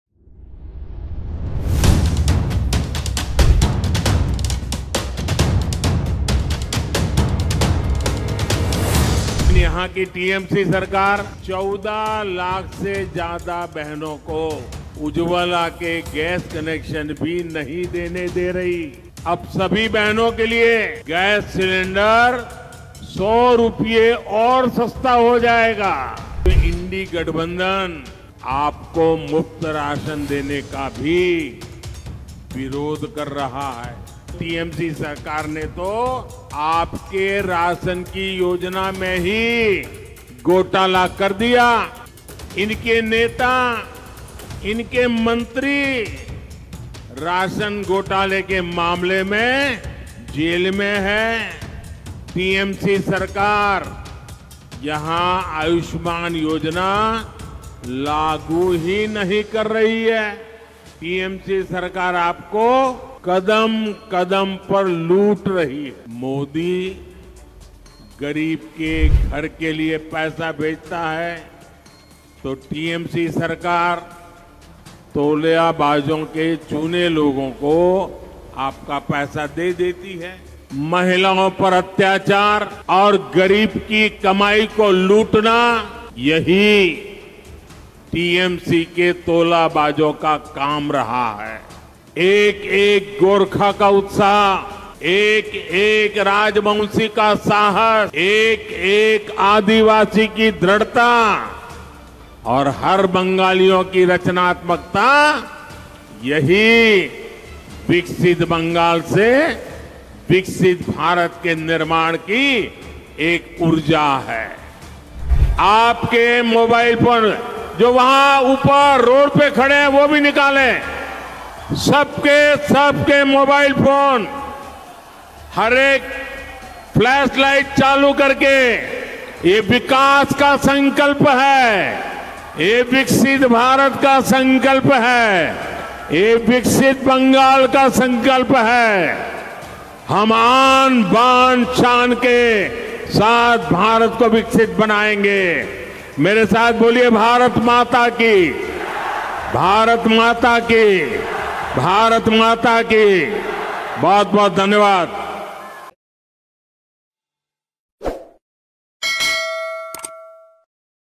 বাংলার কোণে কোণে আওয়াজ উঠেছে - এইবার ৪০০ পার!BanglaChaicheModiThak
শিলিগুড়িতে প্রধানমন্ত্রী শ্রী নরেন্দ্র মোদী জীর বিজয় সংকল্প সভার কিছু ঝলক।

বাংলার কোণে কোণে আওয়াজ উঠেছে - এইবার ৪০০ পার!